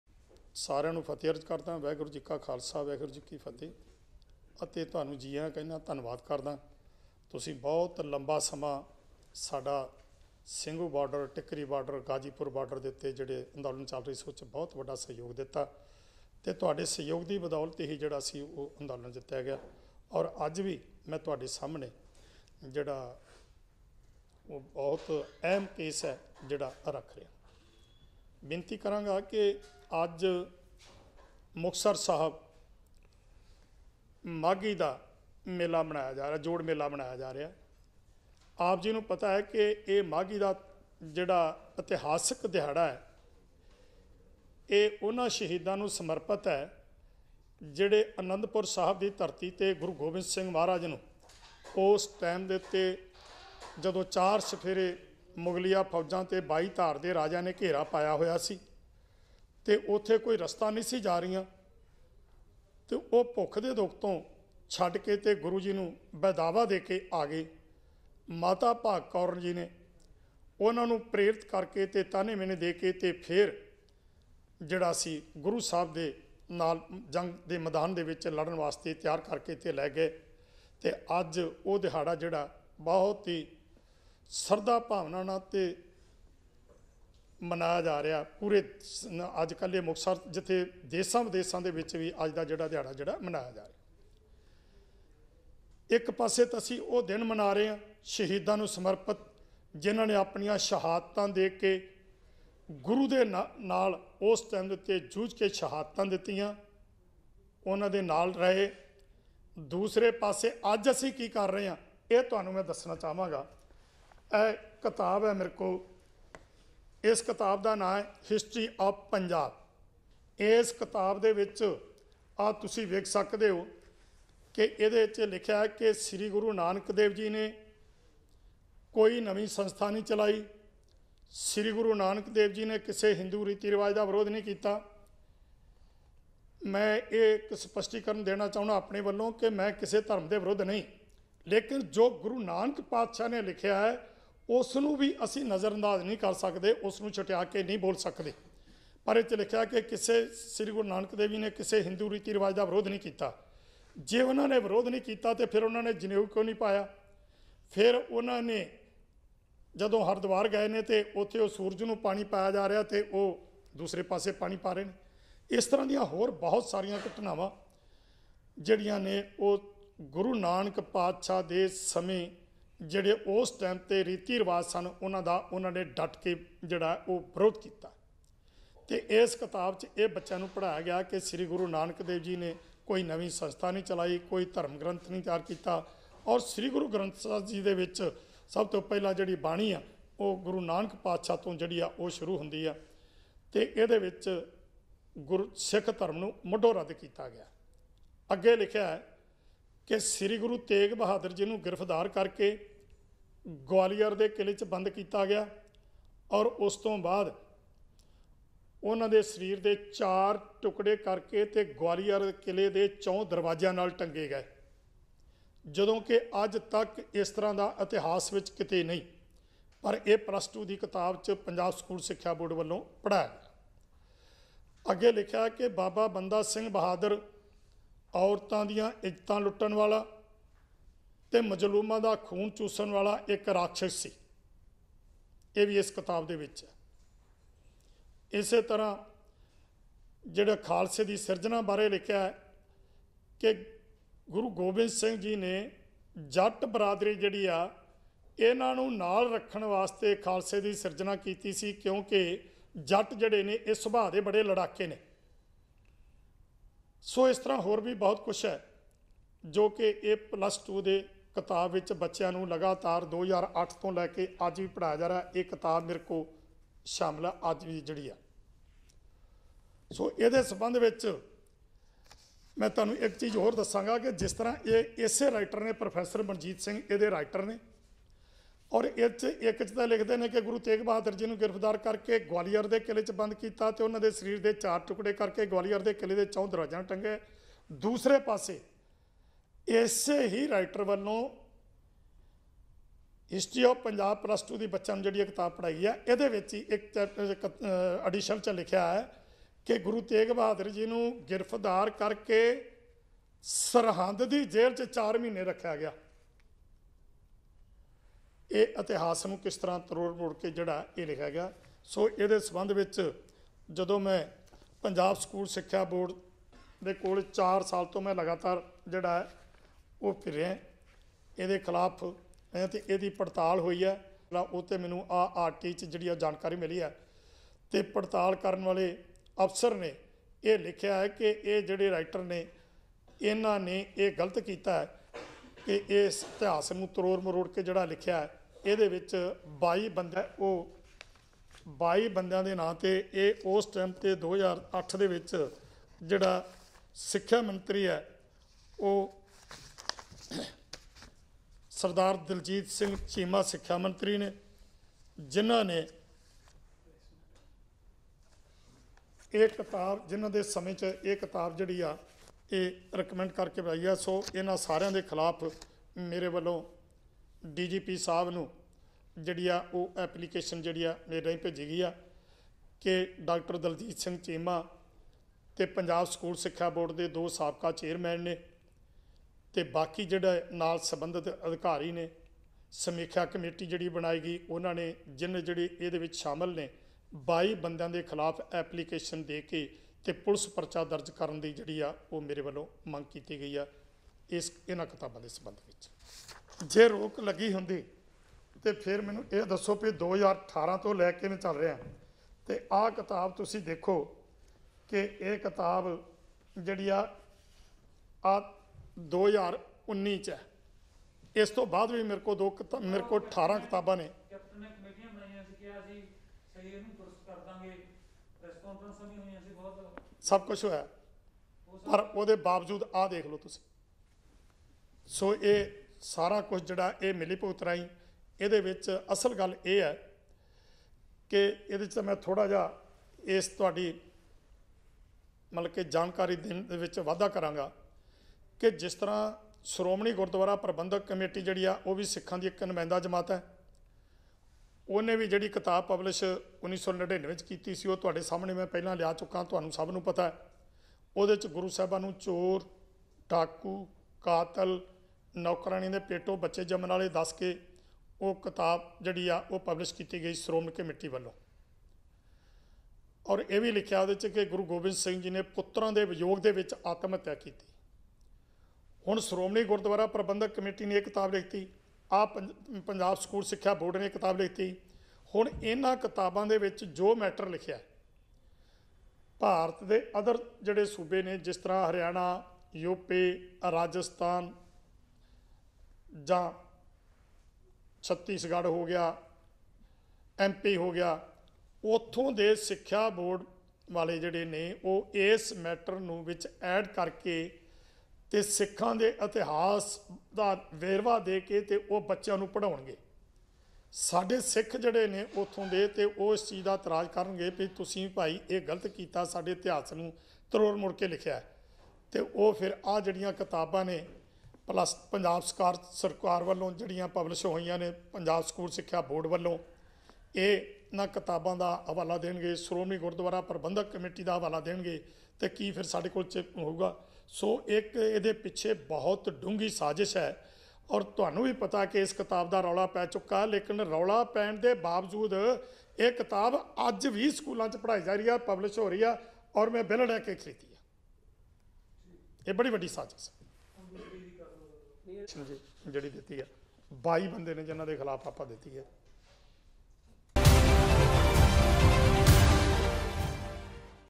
ਸਿੱਖ ਇਤਿਹਾਸ ਨਾਲ ਛੇੜਛਾੜ ਕਰਨ ਵਾਲੇ ਤੇ ਪਰਚਾ ਦਰਜ ਹੋਣਾ ਚਾਹੀਦਾ ਕਿਸਾਨ ਆਗੂ ਬਲਦੇਵ ਸਿੰਘ ਸਿਰਸਾ
ਸਿੱਖ ਇਤਿਹਾਸ ਨਾਲ ਛੇੜਛਾੜ ਕਰਨ ਵਾਲੇ ਤੇ ਪਰਚਾ ਦਰਜ ਹੋਣਾ ਚਾਹੀਦਾ
ਕਿਸਾਨ ਆਗੂ ਬਲਦੇਵ ਸਿੰਘ ਸਿਰਸਾ